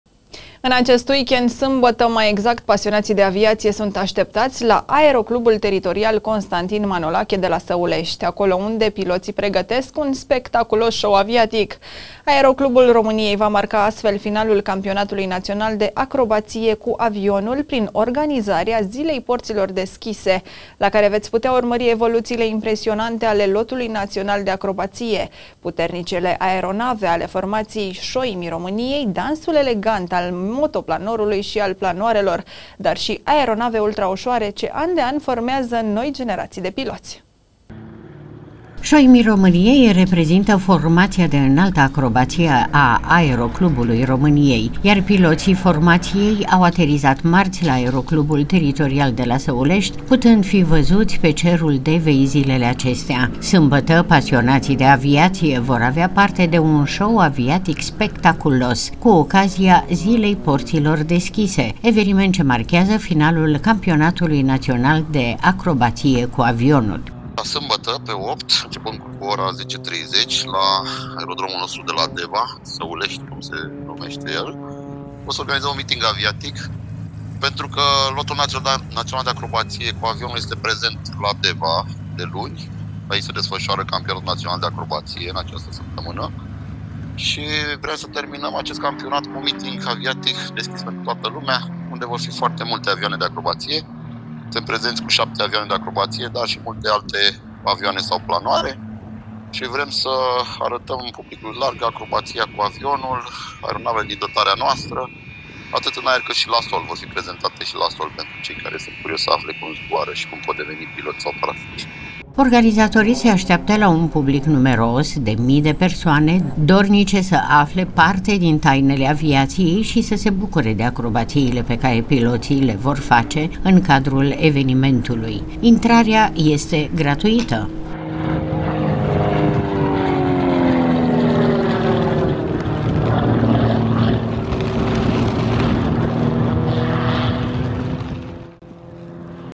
SPECTACOL AVIATIC, LA SĂULEȘTI, CU „ȘOIMII ROMÂNIEI”
MITING AVIATIC CU LOTUL NAȚIONAL DE ACROBAȚIE CU AVIONUL, CARE SE AFLĂ DEJA ÎN JUDEȚ
       În acest weekend, sâmbătă, mai exact, pasionații de aviație sunt așteptați la Aeroclubul Teritorial „Constantin Manolache” de la Săulești, acolo unde piloții pregătesc un spectaculos show aviatic. Aeroclubul României va marca astfel finalul Campionatului Național de Acrobație cu Avionul, prin organizarea „Zilei Porților Deschise”, la care veți putea urmări evoluțiile impresionante ale lotului național de acrobație, puternicele aeronave ale formației „Șoimii României”, dansul elegant al motoplanorului și al planoarelor, dar și aeronavele ultraușoare ce an de an formează noi generații de piloți.